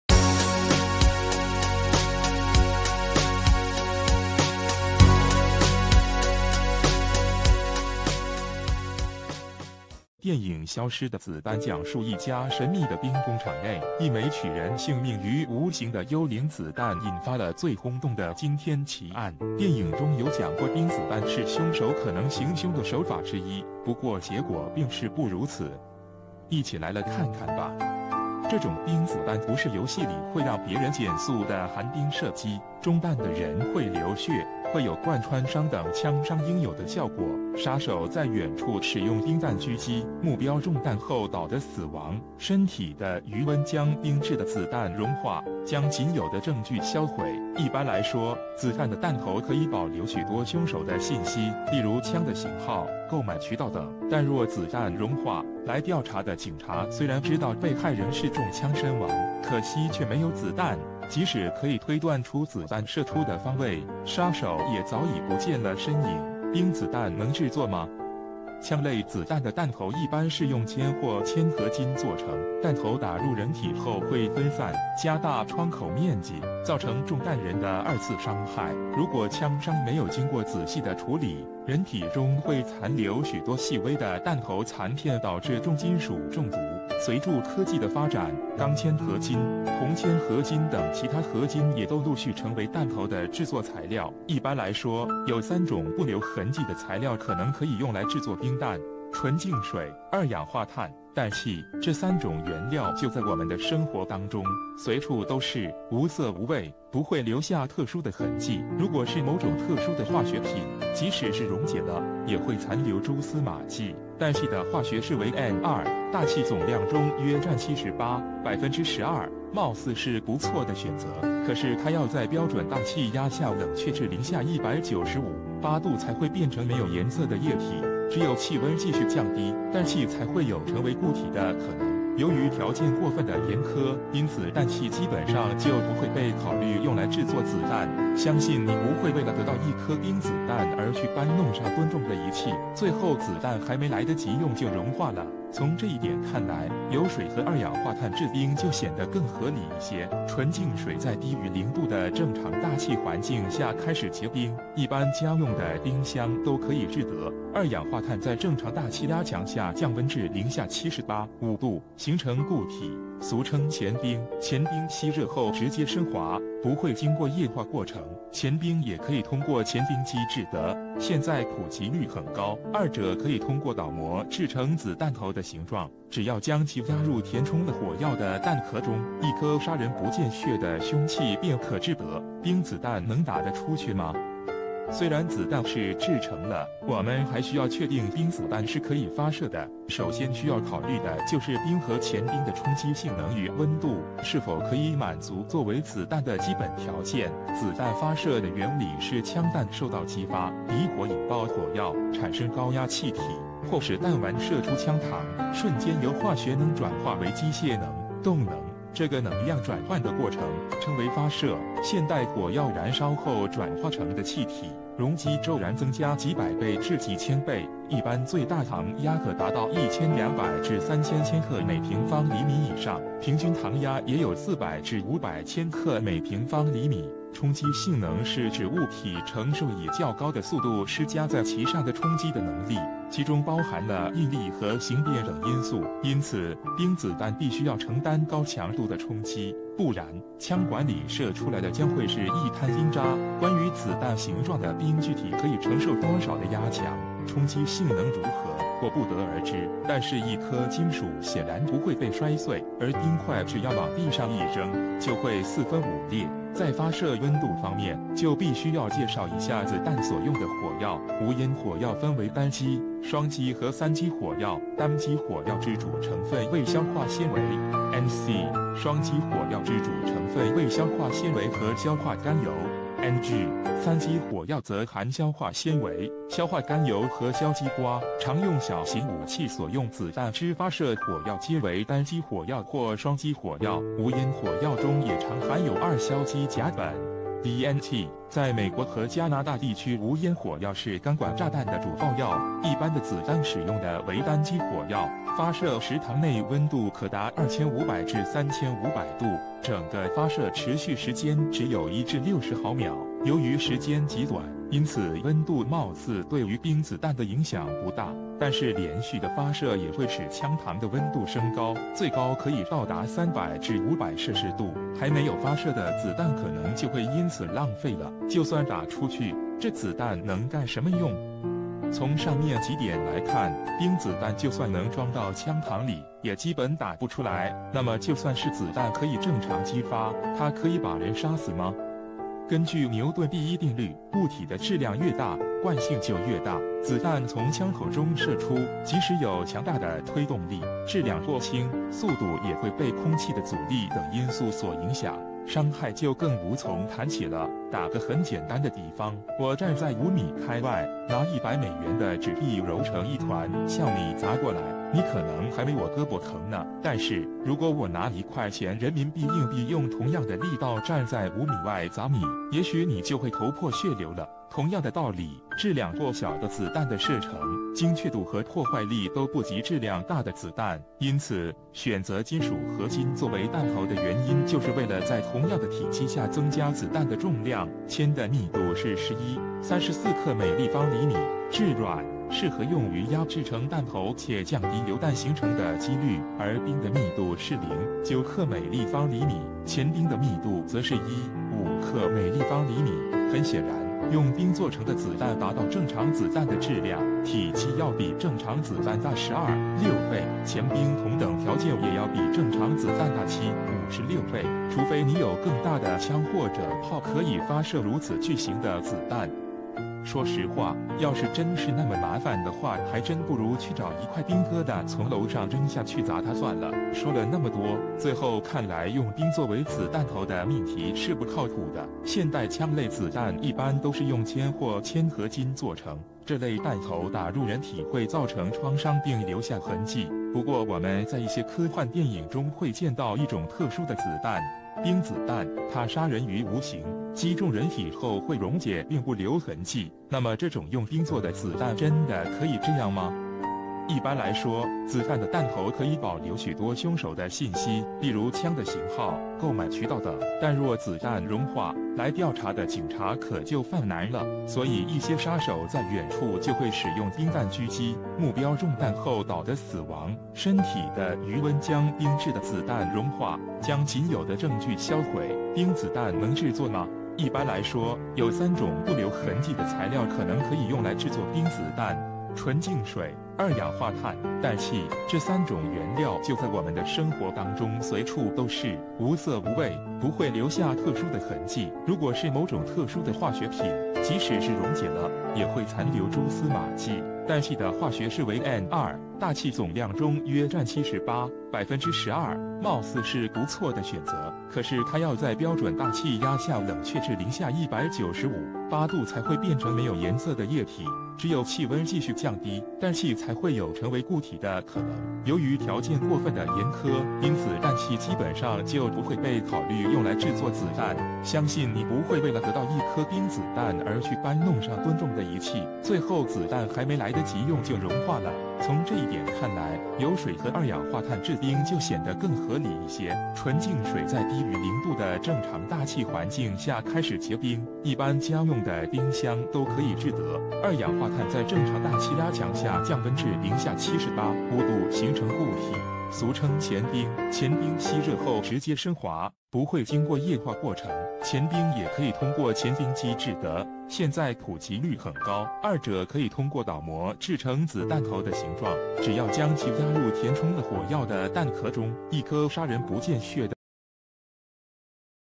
什麼是冰子彈，冰作為子彈頭的命題是不靠譜的
電影《消失的子彈》講述一家神秘的兵工廠內，一枚取人性命於無形的「幽靈子彈」引發了最轟動的驚天奇案。電影中有講過「冰子彈」是兇手可能行兇的手法之一，不過結果並是不如此!一起來了看看吧!

這種「冰子彈」不是遊戲里會讓別人減速的「寒冰射擊」，中彈的人會留血，會有貫穿傷等槍傷應有的效果。殺手在遠處使用冰彈狙擊，目標中彈後倒地死亡，身體的餘溫將冰制的子彈融化，將僅有的證據銷毀。一般來說，子彈的彈頭可以保留許多兇手的信息，例如槍的型號，購買渠道等。但若子彈融化，來調查的警察雖然知道被害人是中槍身亡，可惜卻沒有子彈，即使可以推斷出子彈射出的方位，殺手也早已不見了身影。

冰子彈能製作嗎?

槍類子彈的彈頭一般是用鉛或鉛合金做成，彈頭打入人體後會分散，加大創口面積，造成中彈人的二次傷害。如果槍傷沒有經過仔細地處理，人體中會殘留許多細微的彈頭殘片導致重金屬中毒。隨著科技的發展，鋼鉛合金、銅鉛合金等其它合金也都陸續成為彈頭的製作材料。

一般來說，有三種不留痕跡的材料可能可以用來製作冰彈：純凈水，二氧化碳，氮氣。這三種原料就在我們的生活當中，隨處都是，無色無味，不會留下特殊的痕跡。如果是某種特殊的化學品，即使是溶解了，也會殘留蛛絲馬跡。

氮氣的化學式為n2，大氣總量中約占78.12%，貌似是不錯的選擇。可是它要在標準大氣壓下冷卻至零下195.8℃才會變成沒有顏色的液體，只有氣溫繼續降低，氮氣才會有成為固體的可能。由於條件過份的嚴苛，因此氮氣基本上就不會被考慮用來製作子彈。相信你不會為了得到一顆冰子彈而去搬弄上噸重的儀器，最後子彈還沒來得及用就融化了。

從這一點看來，由水和二氧化碳製冰就顯得更合理一些。純凈水在低於零度的正常大氣環境下開始結冰，一般家用的冰箱都可以製得。二氧化碳在正常大氣壓強下降溫至零下78.5℃，形成固體，俗稱乾冰。乾冰吸熱後直接升華，不會經過液化過程。乾冰也可以通過乾冰機製得，現在普及率很高。二者可以通過倒模製成子彈頭的形狀，只要將其壓入填充了火藥的彈殼中，一顆殺人不見血的兇器便可製得。

冰子彈能打得出去嗎?

雖然子彈是製成了，我們還需要確定「冰子彈」是可以發射的。首先需要考慮的就是冰和乾冰的衝擊性能與溫度，是否可以滿足作為子彈的基本條件。子彈發射的原理是槍彈受到擊發，底火引爆火藥，產生高壓氣體，迫使彈丸射出槍膛，瞬間由化學能轉化為機械能、動能，這個能量轉換的過程，稱為發射。

現代火藥燃燒後轉化成的氣體，容積驟然增加幾百倍至幾千倍，一般最大膛壓可達到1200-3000千克/平方厘米以上，平均膛壓也有400-500千克/平方厘米。衝擊性能是指物體承受以較高的速度施加在其上的衝擊的能力，其中包含了應力和形變等因素。因此，冰子彈必須要承擔高強度的衝擊，不然，槍管里射出來的將會是一灘冰渣。關於子彈形狀的「冰」具體可以承受多少的壓強，衝擊性能如何，我不得而知。但是一顆金屬顯然不會被摔碎，而冰塊只要往地上一扔，就會四分五裂。

在發射溫度方面，就必須要介紹一下子彈所用的火藥。無煙火藥分為單基、雙基和三基火藥，單基火藥之主成分為硝化纖維(nc)，雙基火藥之主成分為硝化纖維和硝化甘油(ng)，三基火藥則含硝化纖維、硝化甘油和硝基胍。常用小型武器所用子彈之發射火藥皆為單基火藥或雙基火藥。無煙火藥中也常含有二硝基甲苯(dnt)，在美國和加拿大地區無煙火藥是鋼管炸彈的主爆藥。

一般的子彈使用的為單基火藥，發射時膛內溫度可達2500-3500℃，整個發射持續時間只有1-60毫秒。由於時間極短，因此溫度貌似對於冰子彈的影響不大。但是連續的發射也會使槍膛的溫度升高，最高可以到達300-500攝氏度，還沒有發射的子彈可能就會因此「浪費」了。

就算打出去，這子彈能幹什麼用?

從上面幾點來看，冰子彈就算能裝到槍膛里，也基本打不出來。那麼就算是子彈可以正常擊發，它可以把人殺死嗎?根據牛頓第一定律，物體的質量越大，慣性就越大。子彈從槍口中射出，即使有強大的推動力，質量過輕，速度也會被空氣的阻力等因素所影響，傷害就更無從談起了。

打個很簡單的比方，我站在5米開外，拿100美元的紙幣揉成一團向你砸過來，你可能還沒我胳膊疼呢。但是，如果我拿1塊錢人民幣硬幣用同樣的力道站在5米外砸你，也許你就會頭破血流了。同樣的道理，質量過小的子彈的射程、精確度和破壞力都不及質量大的子彈。因此，選擇金屬合金作為彈頭的原因就是為了在同樣的體積下增加子彈的重量。

鉛的密度是11.34克/立方厘米，質軟，適合用於壓製成彈頭且降低流彈形成的機率。而冰的密度是0.9克/立方厘米，乾冰的密度則是1.5克/立方厘米。很顯然，用冰做成的子彈達到正常子彈的質量，體積要比正常子彈大12.6倍，乾冰同等條件也要比正常子彈大7.56倍。除非你有更大的槍或者炮可以發射如此「巨型」的子彈……說實話，要是真是那麼麻煩的話還真不如去找一塊冰疙瘩從樓上扔下去砸他算了。

說了那麼多，最後看來用冰作為子彈頭的命題是不靠譜的。